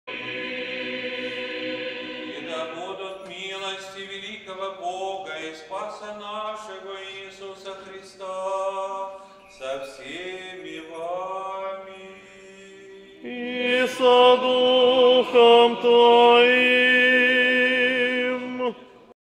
Да будут Милости Великого Бога и Спаса нашего Иисуса Христа со всеми вами .
3 июня 2018 года, в Неделю 1-ю по Пятидесятнице, Всех святых, митрополит Минский и Заславский Павел, Патриарший Экзарх всея Беларуси, совершил Божественную литургию в Свято-Духовом кафедральном соборе города Минска.

Его Высокопреосвященству сослужили: управляющий делами Минской Экзархии архимандрит Сергий (Брич); секретарь Минской епархии протоиерей Андрей Волков и клирики кафедрального собора.

Проповедь перед Причастием произнес клирик Свято-Духова кафедрального собора протодиакон Вадим Зарьков.

По заамвонной молитве митрополит Павел и сослужащие священнослужители совершили славление празднику.

По окончании богослужения владыка Павел поздравил прихожан и духовенство собора с праздником и обратился ко всем присутствующим со словом назидания.

Патриарший Экзарх поздравил причастников с принятием Святых Христовых Таин и преподал всем архипастырское благословение.

Именинникам было провозглашено многолетие, и митрополит Павел вручил им просфоры и розы.